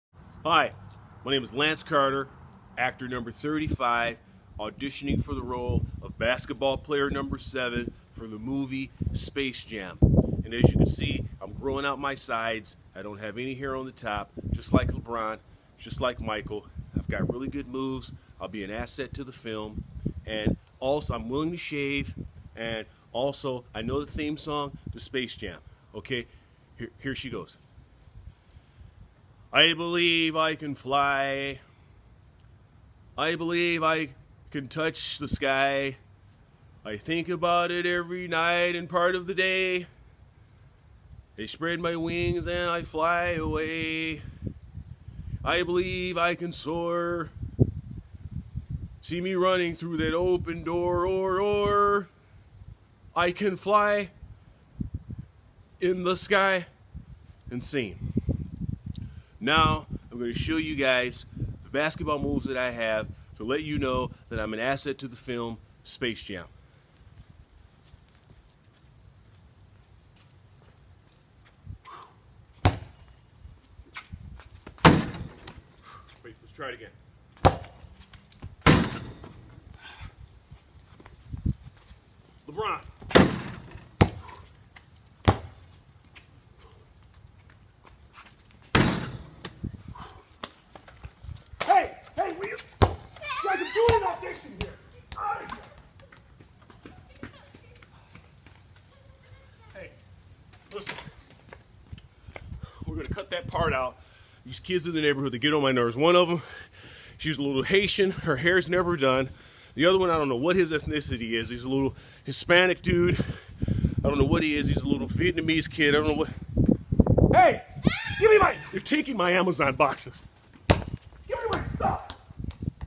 Lance Carter auditions for the movie Space Jam
Lance the Struggling Actor can’t catch a break.